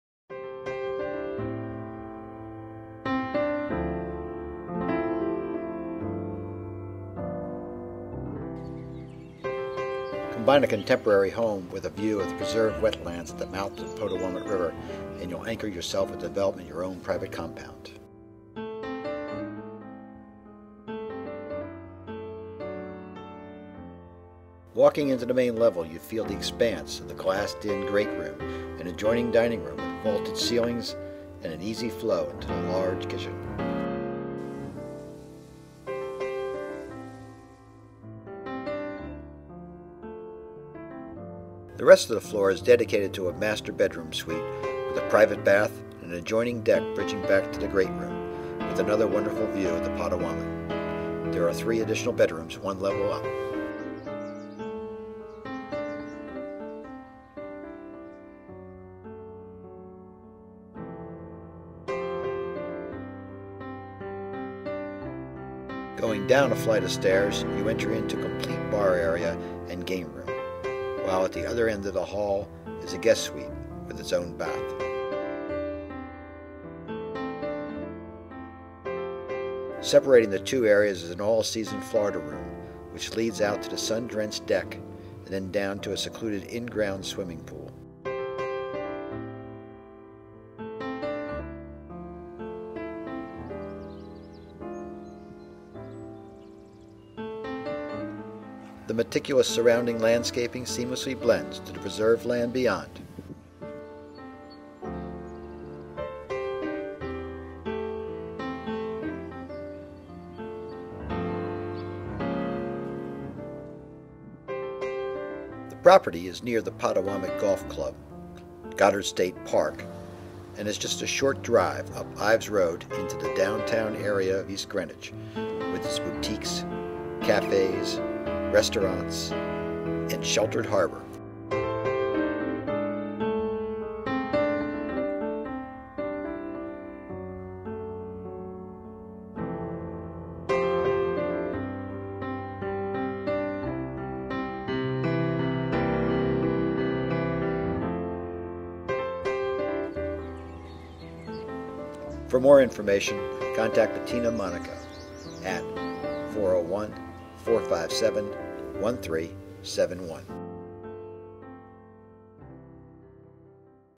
2 Netop Court - Warwick, RI
Combine a contemporary home with a view of preserved wetlands at the mouth of the Potowomut River and you'll anchor yourself with the development of your own private compound.

Walking into the main level you feel the expanse of the glassed-in great room and adjoining dining room with vaulted ceilings and an easy flow into the large kitchen.

The rest of the floor is dedicated to a master bedroom suite with a private bath and an adjoining deck bridging back to the great room with another view of the Potowomut.  There are three additional bedrooms one level up.

Going down a flight of stairs you enter into a complete bar area and game room, while at the other end of the hall is a guest suite with its own bath.  Separating the two areas is an all season Florida room, which leads out to the sun-drenched deck and then down to a secluded in-ground swimming pool.  The meticulous surrounding landscaping seamlessly blends to the preserved land beyond.

The property is near the Potowomut golf club, Goddard state park and is just a short drive up Ives Road into the downtown area of East Greenwich with its boutiques, cafes, restaurants, and sheltered harbor.